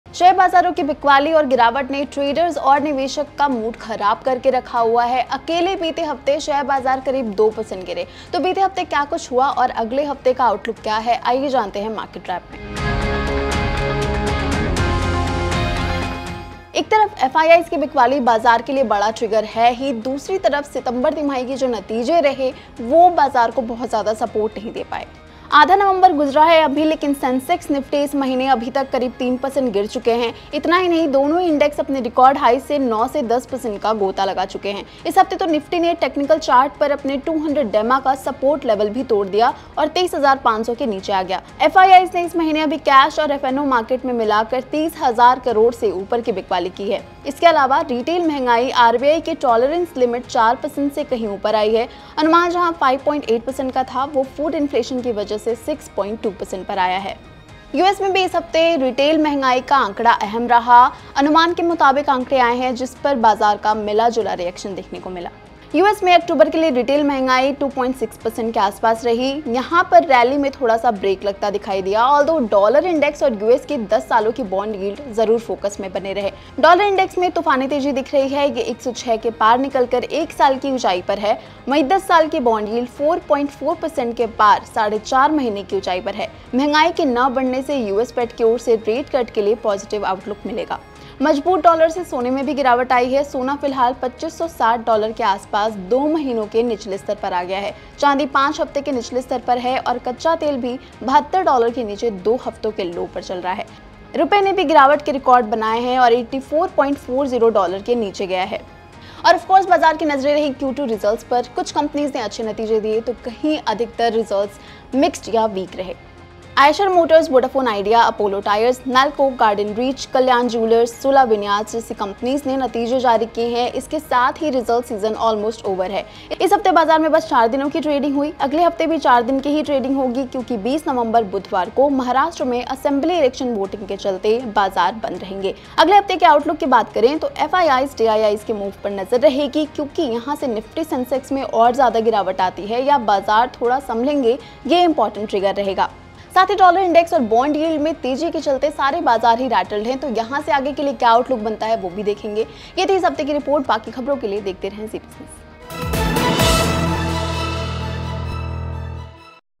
Market Wrap: बॉटम पर बॉटम, कहां तक गिरे शेयर बाजार?
शेयर बाजार में पूरे हफ्ते की उतार-चढ़ाव और गिरावट के बाद फ्लैट क्लोजिंग के साथ हफ्ता खत्म हुआ. दिन भर वॉलेटिलटी के बाद बेंचमार्क इंडेक्स सपाट बंद हुए. बाजार में लगातार छह दिनों की गिरावट रही. इस हफ्ते क्या बड़े ट्रिगर्स रहे, और अगले हफ्ते क्या फोकस में रहेगा, जानें Market Wrap में